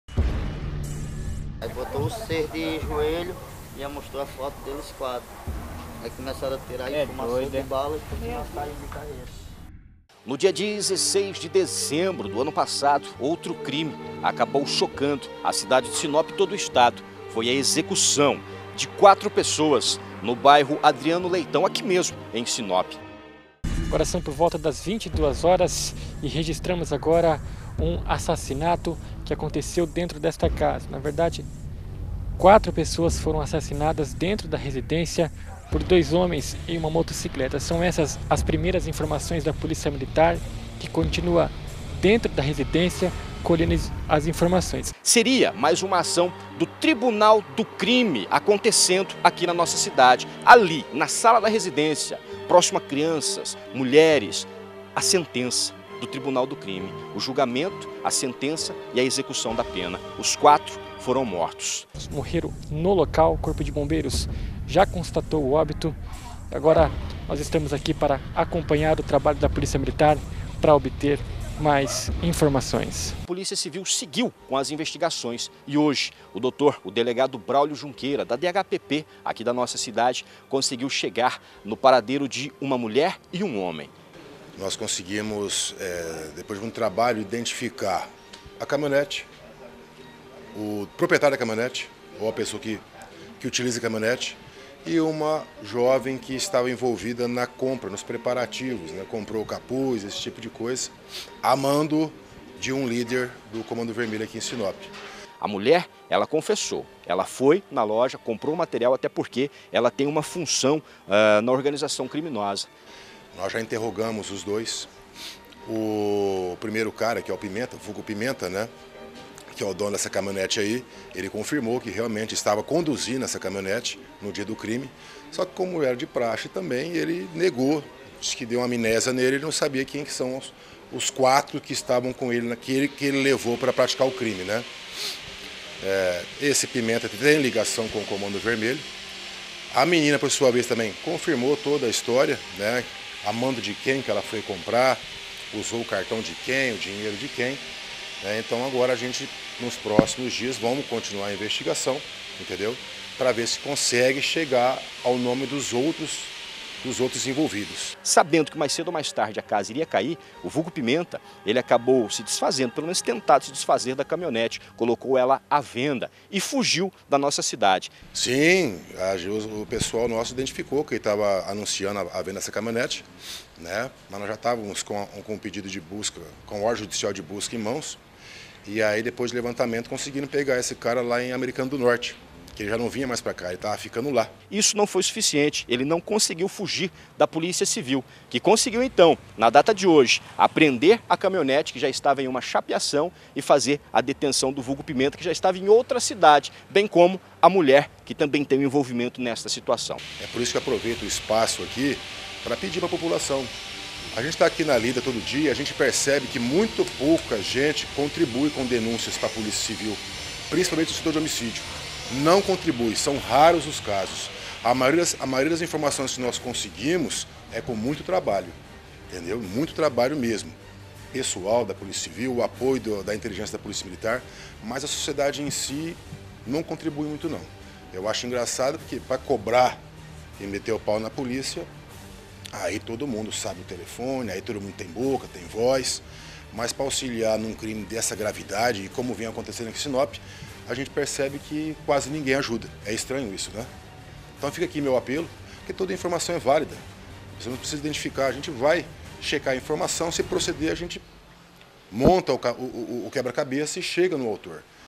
SINOP - POLÍCIA CIVIL PRENDE DOIS SUSPEITOS DE ENVOLVIMENTO NA CHACINA OCORRIDA EM DEZEMBRO
Matéria exibida na TV Cidade Verde/MT
Reportágem: Fernando Itamir
Imagens: